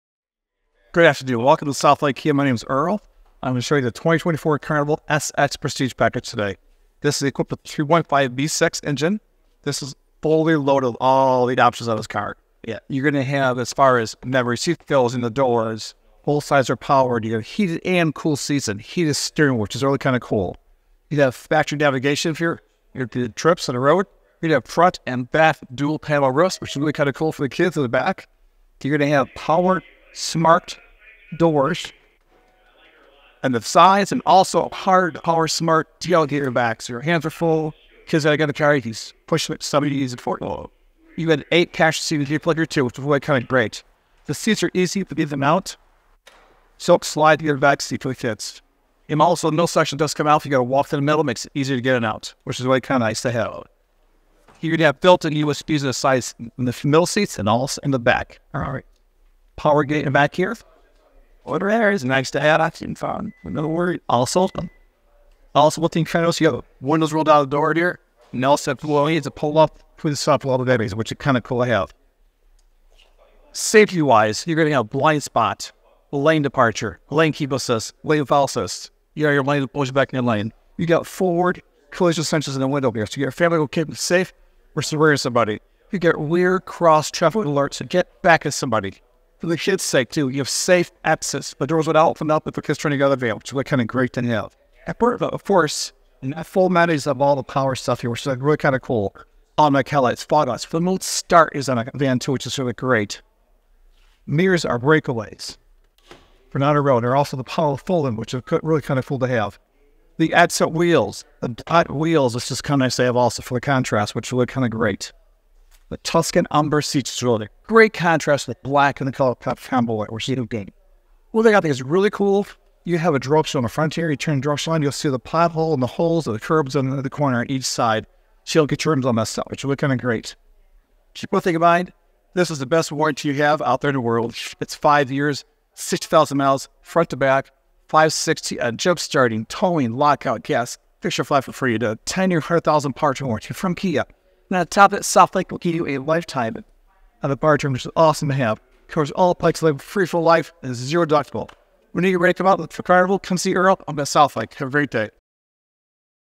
2024 Kia Carnival | Southlake Kia
The 2024 Kia Carnival was built with your convenience in mind! Check out all its features here today and come into Southlake Kia for a test drive!

4191 E. Lincoln HWY
Merrillville, IN 46410